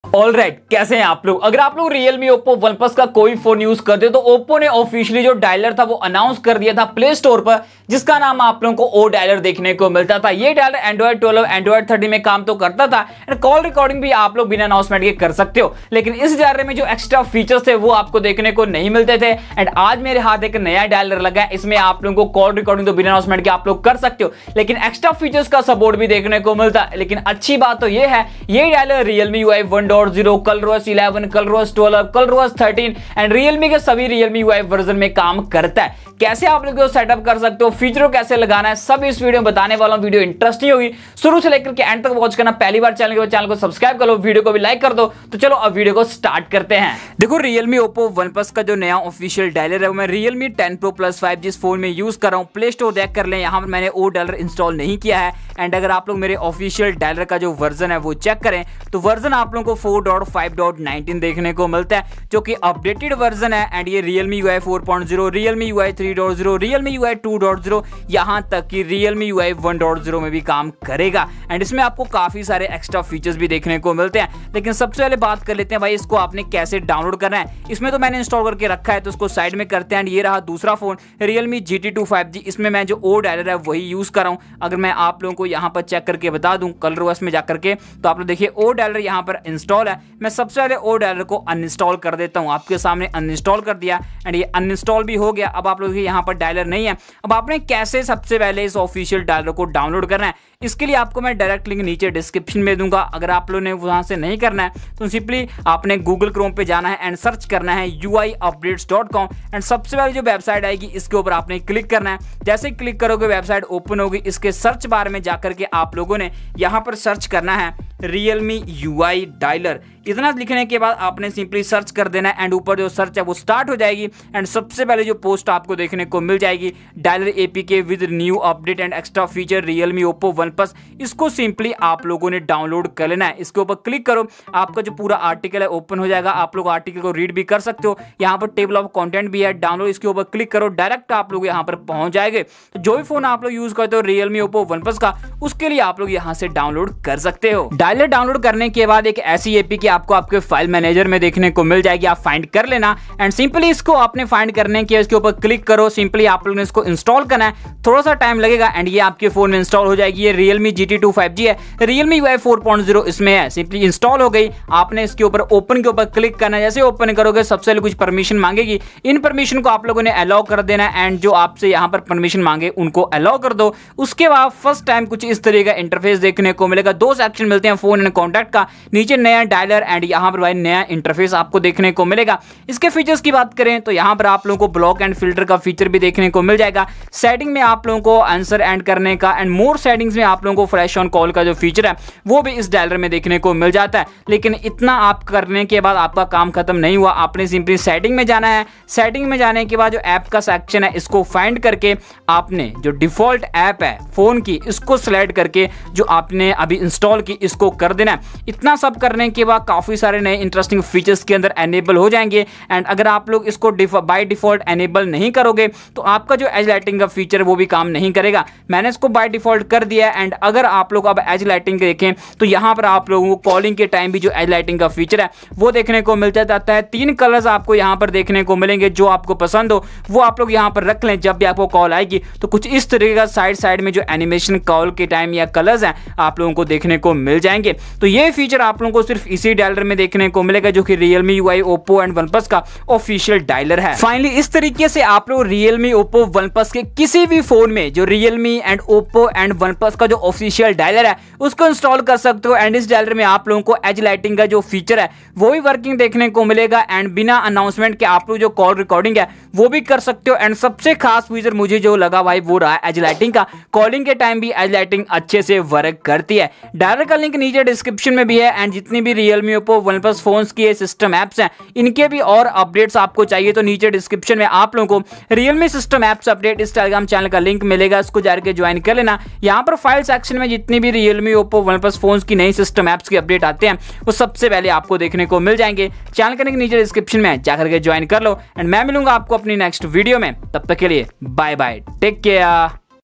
Realme UI Official Dialer New Update | Edge Lighting Features | Call Recording Without Announcement🔥
Topic Covered-
1- realme UI 4.0
2- realme UI official Dialer
3- realme UI Call recording without announcement
4- realme Google dialer to official realme dialer